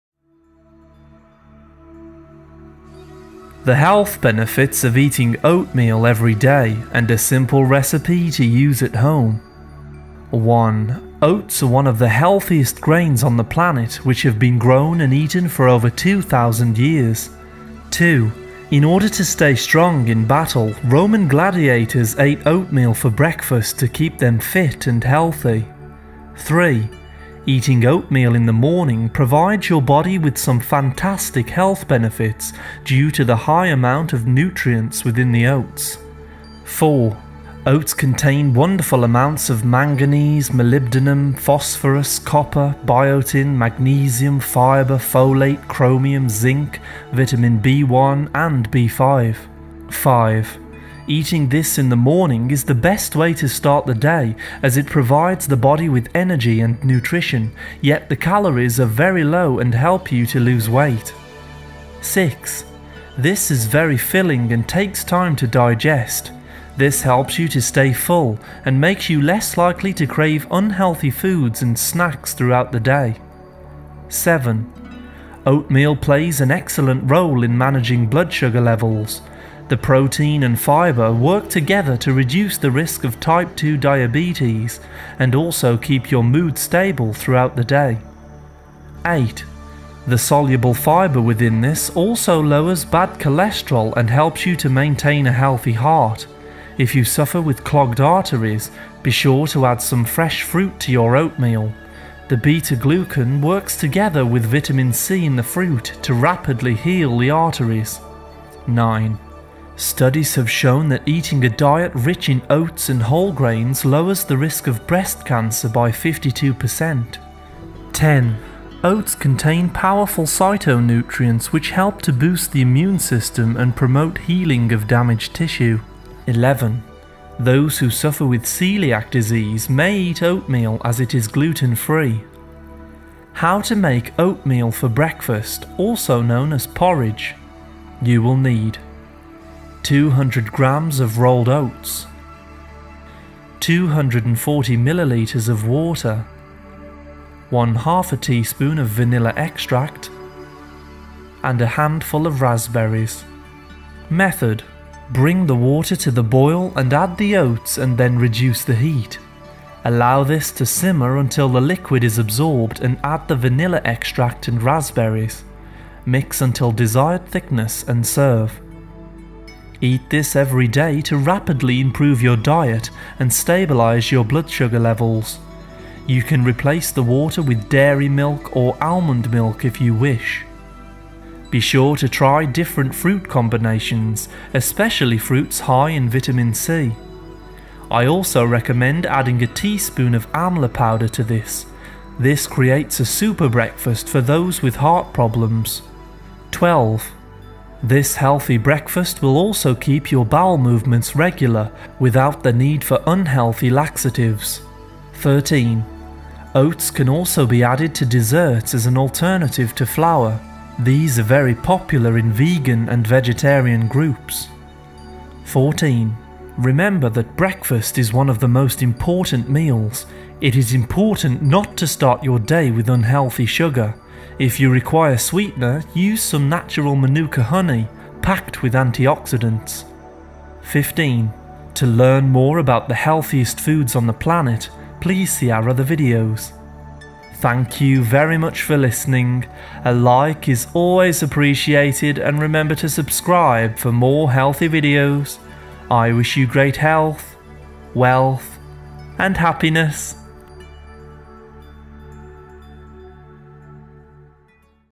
Oatmeal Everyday (Benefits)
The health benefits of eating oatmeal every day. [CC Available]

In today’s video we discuss the many benefits of oatmeal, and how introducing this into your daily diet will increase your health. This super grain was used by Roman gladiators to promote strength and vigor. We recommend eating oatmeal every morning for the best effects as this has been shown to stabilise blood sugar levels, reduce the risk of heart disease, boost energy levels and cause natural weight loss.

Music From: Purple Planet